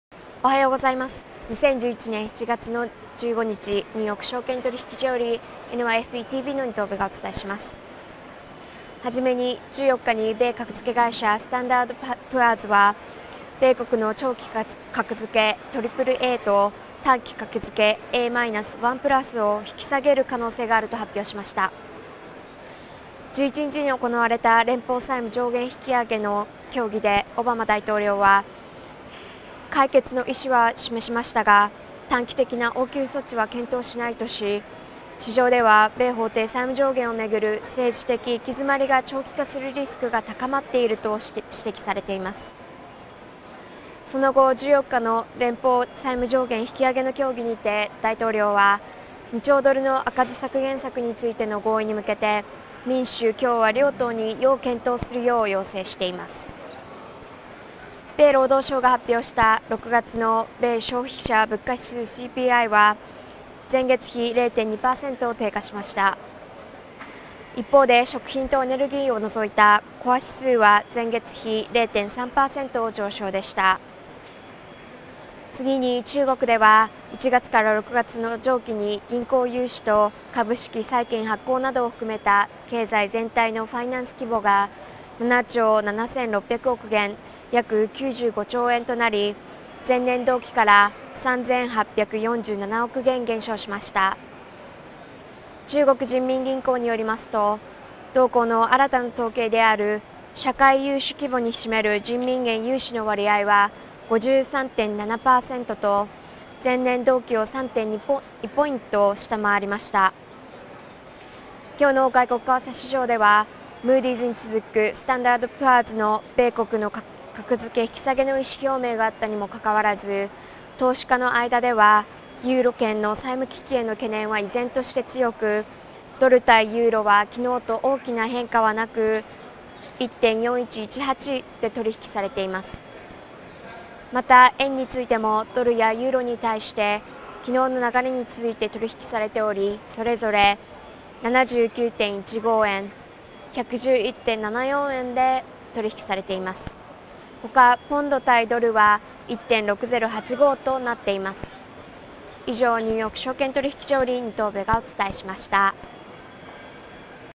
外国為替市場（NY）：米国格付け引き下げでもドル耐える
今日の外国為替市場では、ムディーズに続くスタンダード＆プアーズの米国の格付け引き下げの意思表明があったにも関わらず、投資家の間ではユーロ圏の債務危機への懸念は依然として強く、ドル対ユーロは昨日と大きな変化なく取引されています。